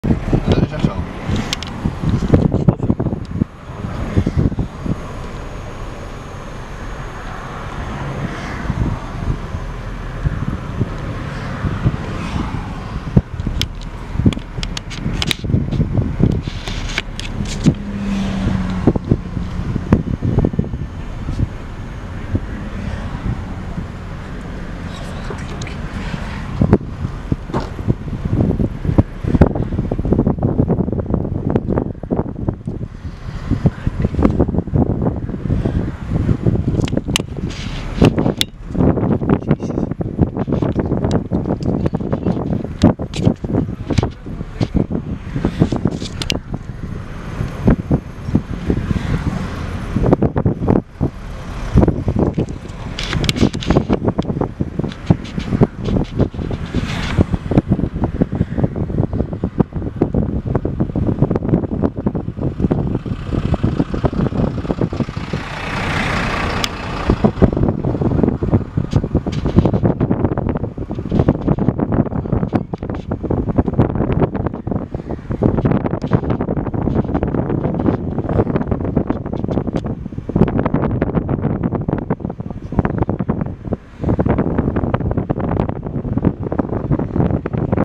Ongeval A1 Bathmen
Bathmen - rond middernacht werden de hulpdiensten gealarmeerd voor een ongeval met meerdere voertuigen. Ter plaatse bleek het te gaan om een ongeval met 3 personen auto's waarvan 1 op het dak tot stilstand is gekomen. Bij het ongeval zijn drie personen met onbekend letsel naar het ziekenhuis overgebracht. De politie doet onderzoek naar de toedracht van het ongeval